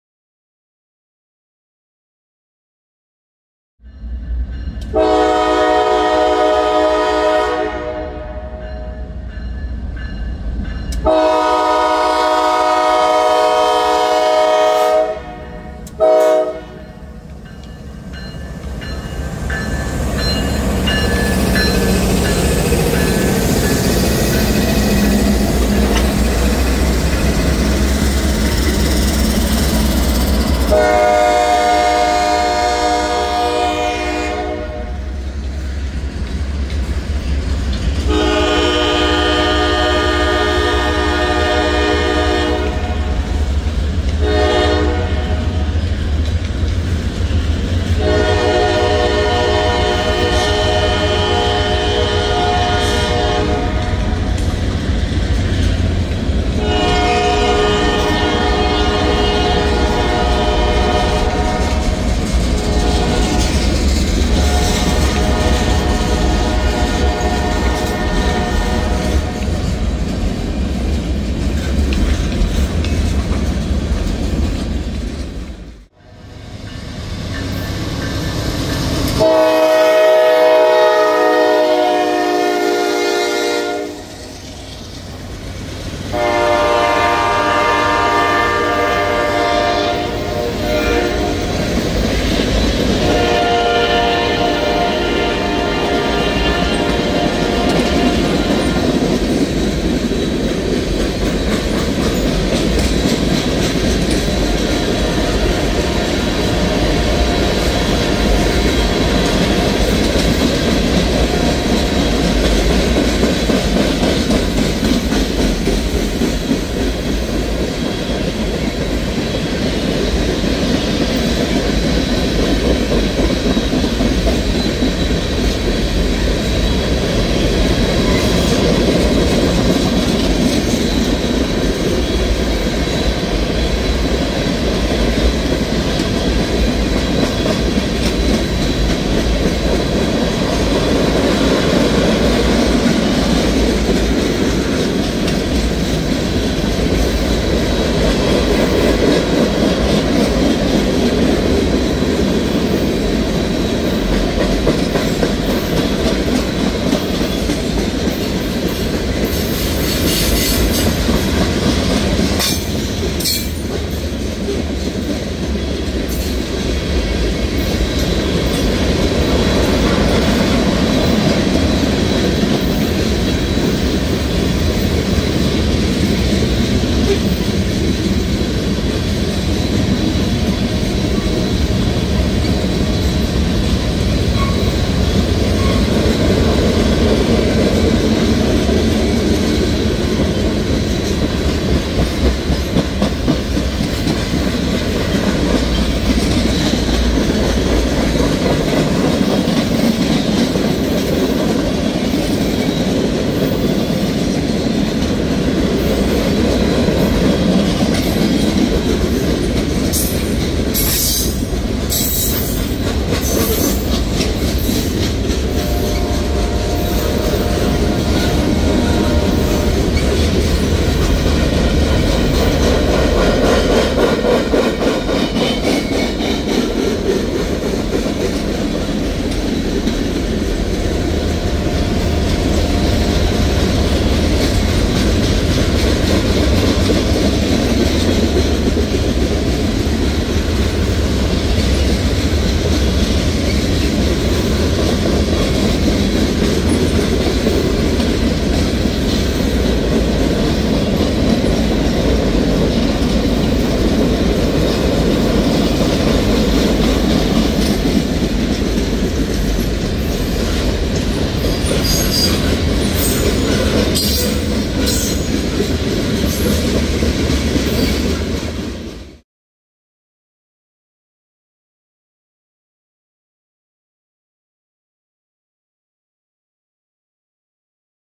Norfolk Southern 4000 DC to AC (Blue) Norfolk Southern 192 Manifest Train
Early Saturday Morning Norfolk Southern 192 NS Heritage Blue 4000 DC to AC Conversion Tankers and Mixed Freight

Northbound on the Columbia District Sub

Norfolk Southern 4743 Lead
Norfolk Southern Heritage Blue 4000

0:00 Intro
0:05 Downtown Augusta 2:45 a.m.
1:15 Graniteville Sc 3:25 a.m.